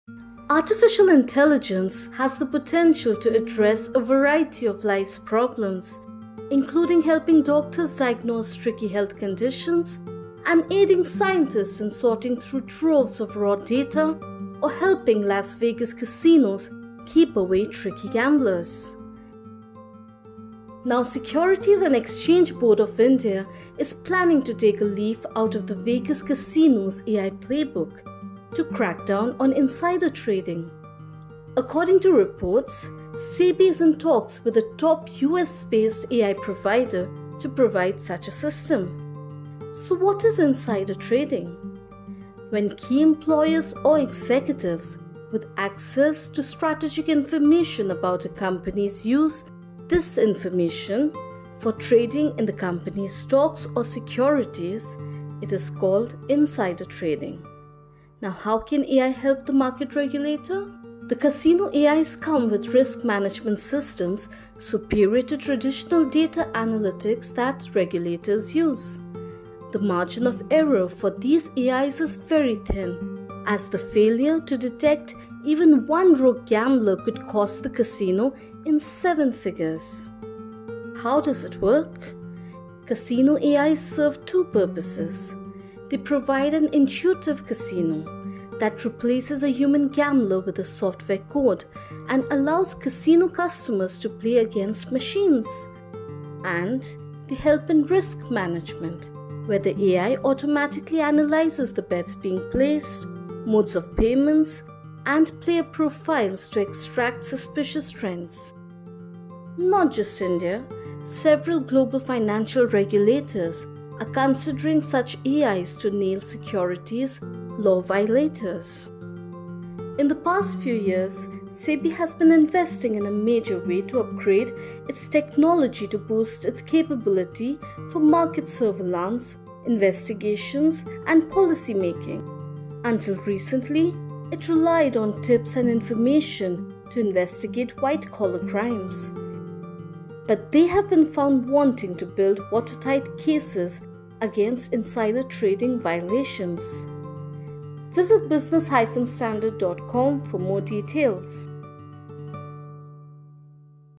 How Las Vegas casino AI can help Sebi stamp out insider trading: Explained
Artificial intelligence has the potential to address a variety of life's problems, including helping doctors diagnose tricky health conditions and aiding scientists in sorting through troves of raw data or helping Las Vegas casinos keep away tricky gamblers.

Now, Securities and Exchange Board of India is planning to take a leaf out of the Vegas Casinos' AI playbook to crack down on insider trading. Watch the video to know-how.